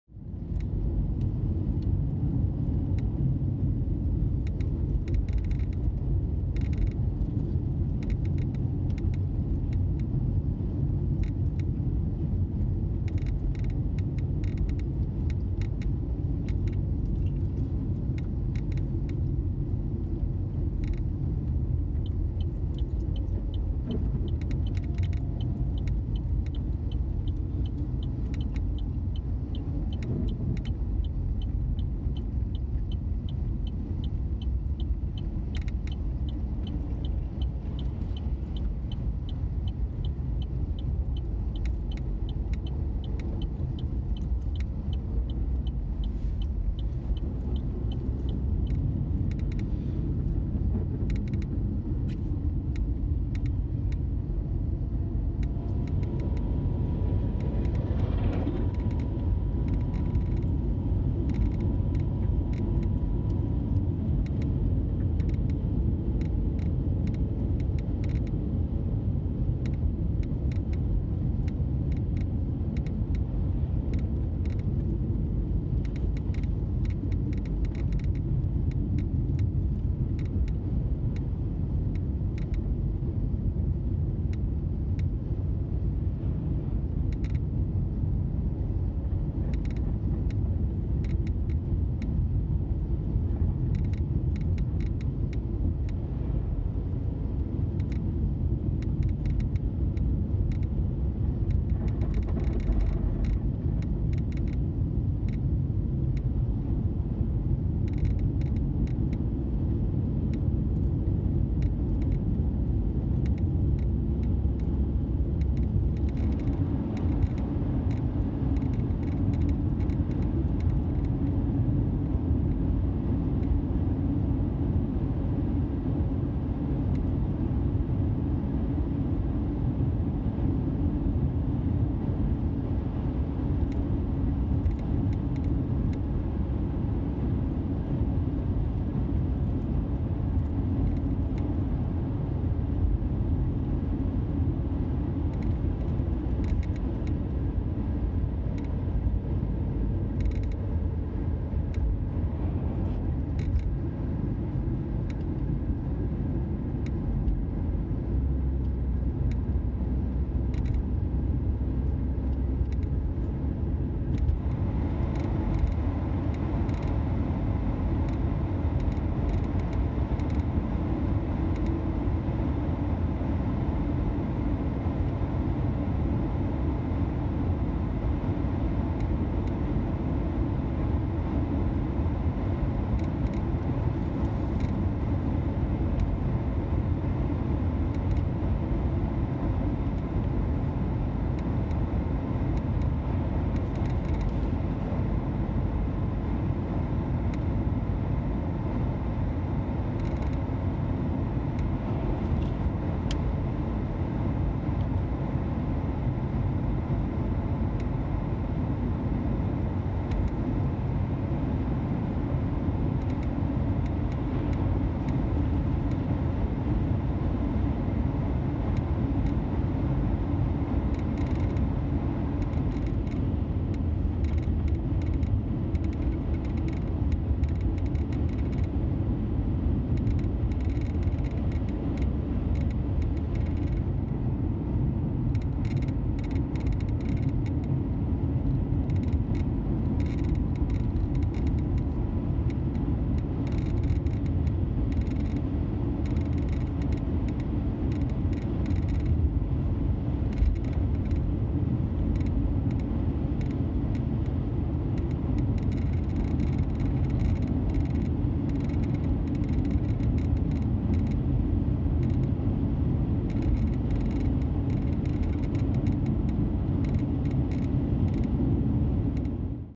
Driving Highway 3 in the Northwest Territories
Shot on a Google Pixel in several different areas in the NorthwestTerritories on Jan. 30, 2019.